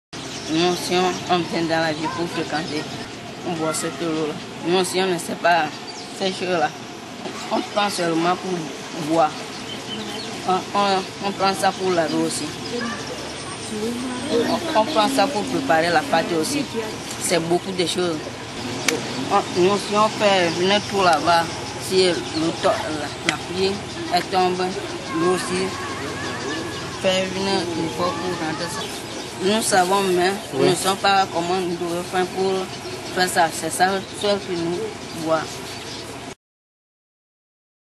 Problèmes d'eau potable
Les populations du village de Zouvi ont un besoin urgent d'eau potable car l'eau qu'elles consomment au moment où Les Amis de la Terre Togo les a visitées n'est pas bonne même pour les animaux.